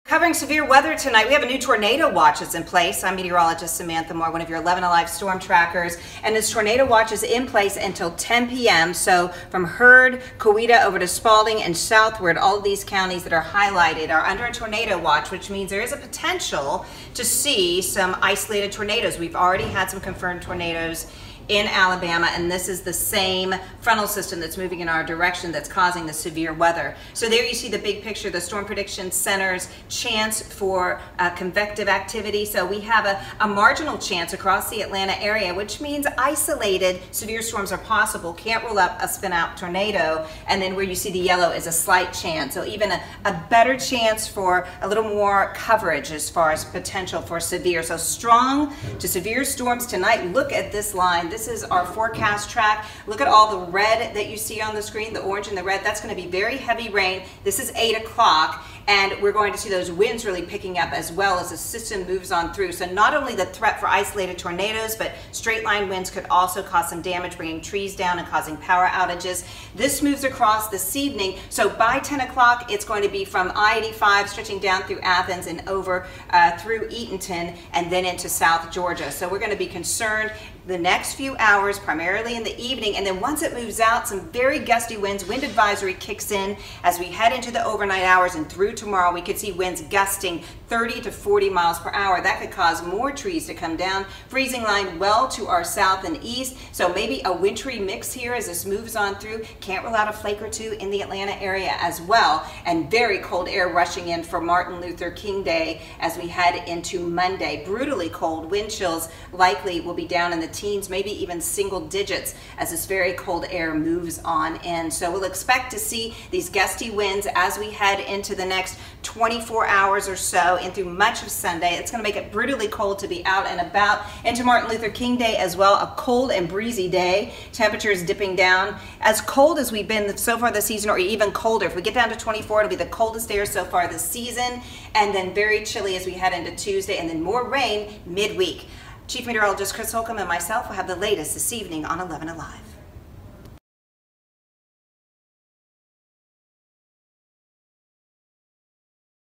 Tornado watch on the southside
Tornado watch until 10pm south of Atlanta.